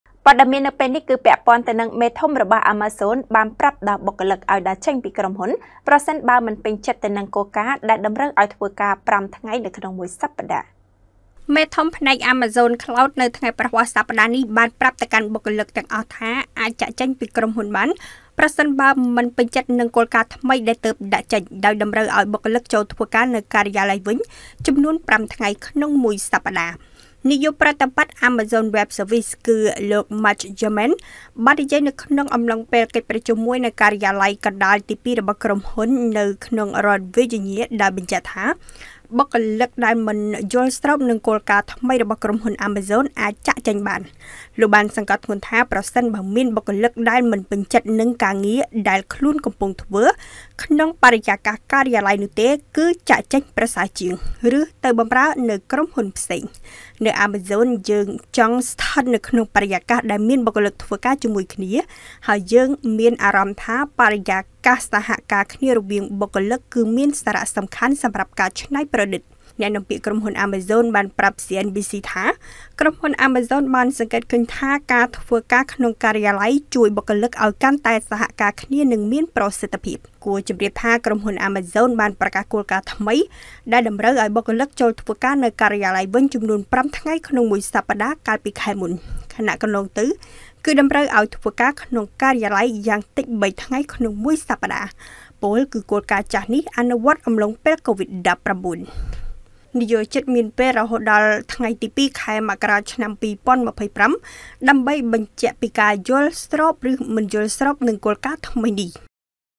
មេធំ Amazon Cloud ប្រាប់ឲ្យបុគ្គលិកដើរចេញពីក្រុមហ៊ុន ប្រសិនបើមិនពេញចិត្តនឹងគោលការណ៍ចូលធ្វើការ...
មេធំ Amazon Cloud ប្រាប់ឲ្យបុគ្គលិកដើរចេញពីក្រុមហ៊ុន ប្រសិនបើមិនពេញចិត្តនឹងគោលការណ៍ចូលធ្វើការ៥ថ្ងៃក្នុងមួយសប្តាហ៍នៅការិយាល័យ
Fresh News Plus